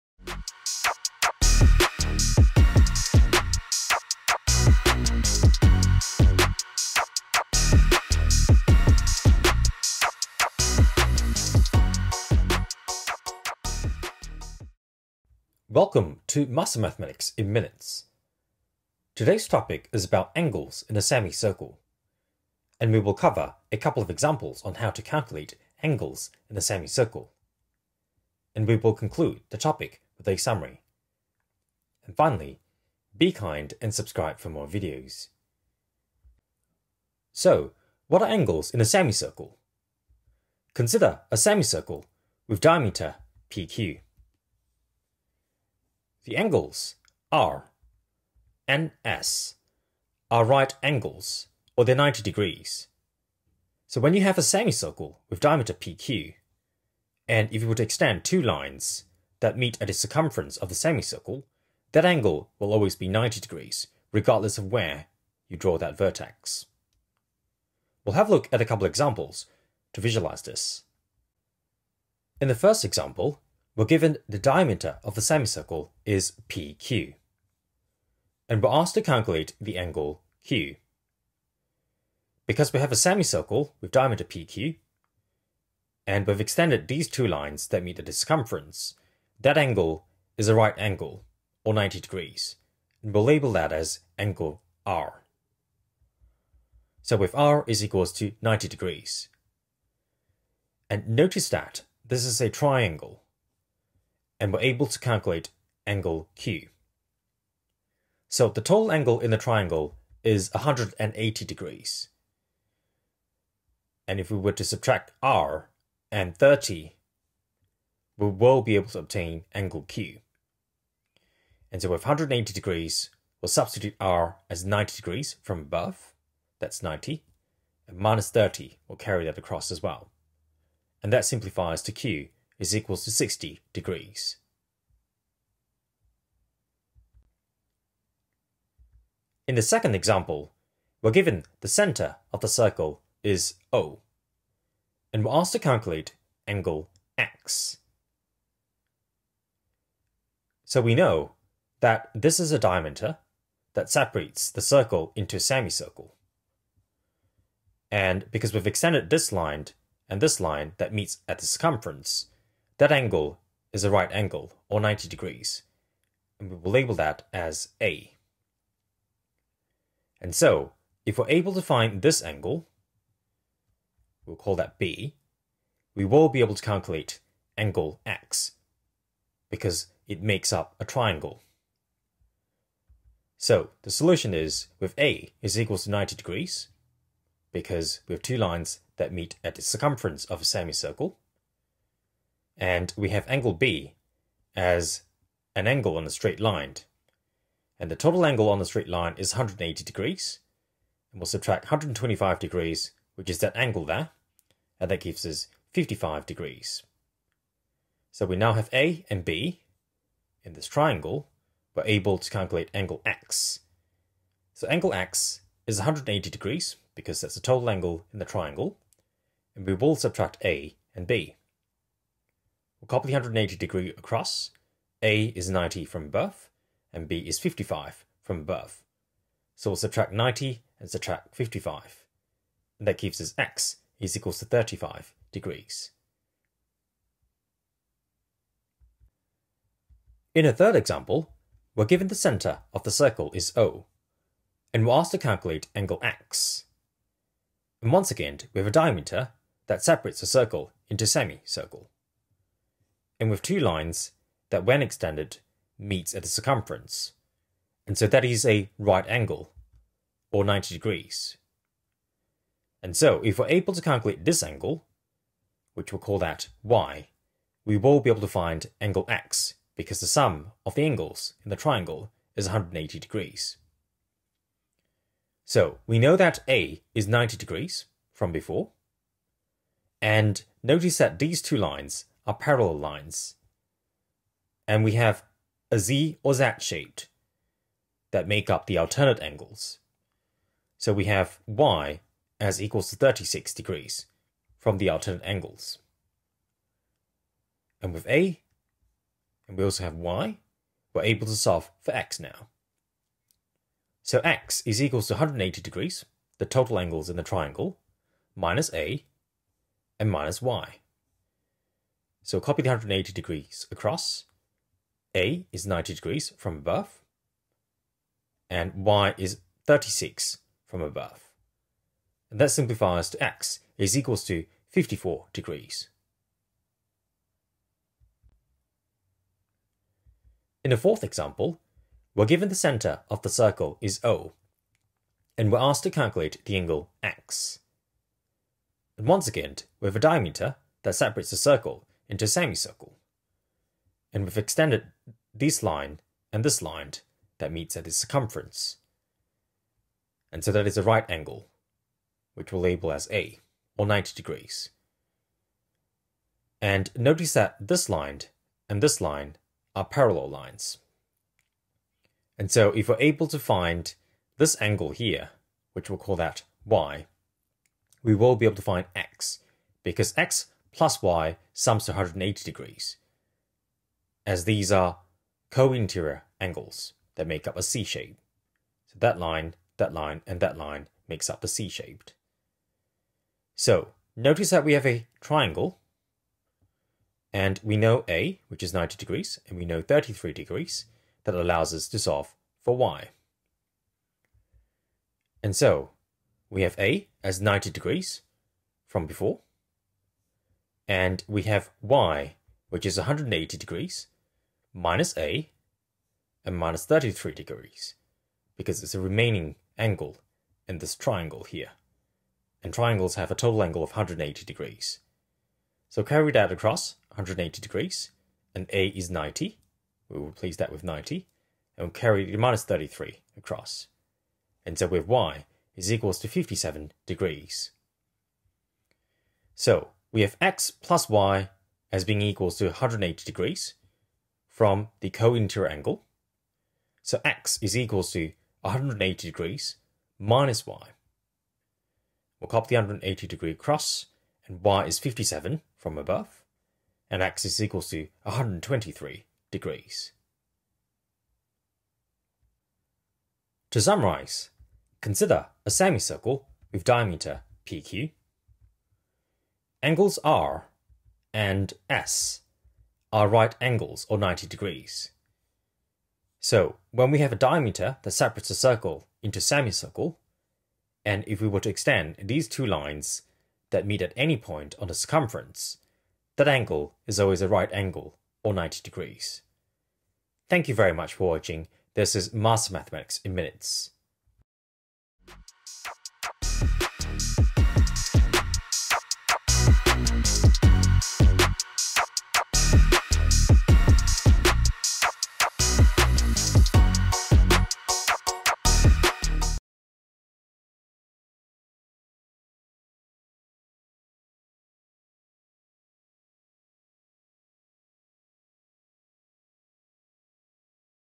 How to calculate the angles in a semicircle?
Master Mathematics in Minutes | Geometry | Angles in a Semicircle

We explore the angles in a semicircle with examples and solutions.

Chapters
00:00 What will you master?
00:36 Angles in a semicircle
01:11 First example
02:17 Second example
03:59 Third example
05:28 Fourth example
07:32 Summary